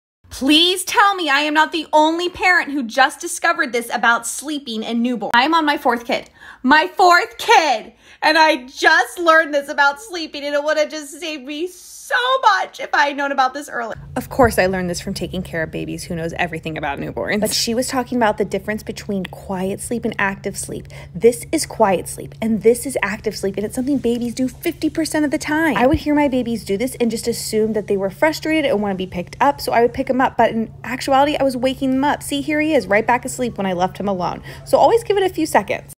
Newborn Sleep Tips: Active Sleep vs Quiet Sleep
Knowing the difference between quiet sleep and active sleep will save you some zzz’s during the newborn stage!

Every time my babies fussed during their sleep I would pick them up. Even though they weren’t crying, I assumed they were frustrated and wanted to be held. Little did I know they were in a state of active sleep and I was waking them up when I did that!

There is a power to the pause when it comes to newborn sleep. If your baby isn’t crying and doesn’t seem to be in distress, give it a minute before picking them up. They may be in an active sleep cycle!

I wish this was something I learned about sooner! My 4th baby sleeps so much better than my other three and I know a lot of that has to do with me not disturbing his sleep when he is in an active cycle!